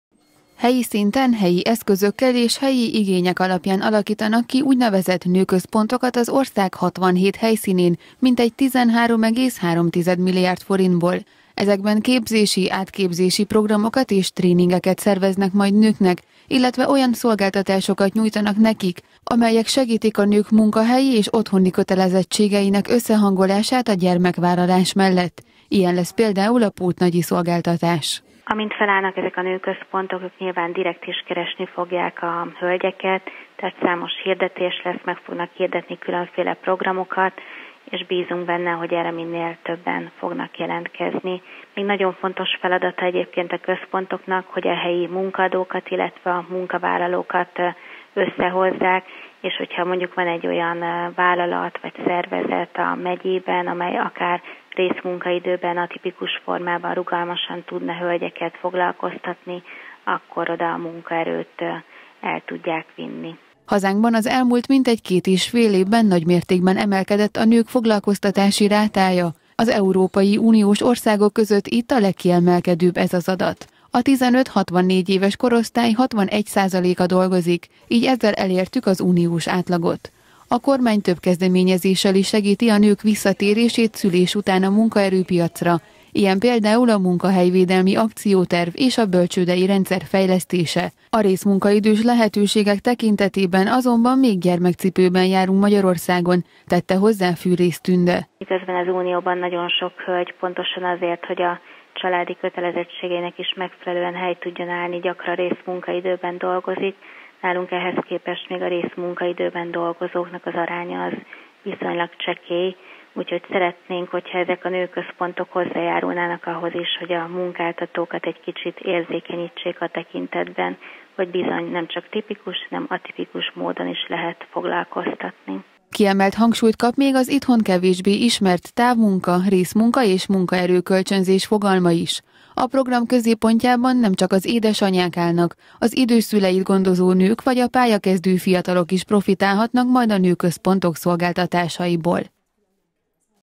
Nő-Köz-Pont - Kölcsey Tv
67 Női Információs és Szolgáltató Központ kezdi meg működését országszerte jövőre.
Ezek a nők munkaerő-piaci elhelyezkedését segítik majd helyi szinteken, kialakításukra összesen 13,3 milliárd forintos forrás áll rendelkezésre. A tervek szerint legalább 34 ezer nőnek segíthetnek majd többek között a munkahelykeresésben, önfoglalkoztatásban és karriertervezésben.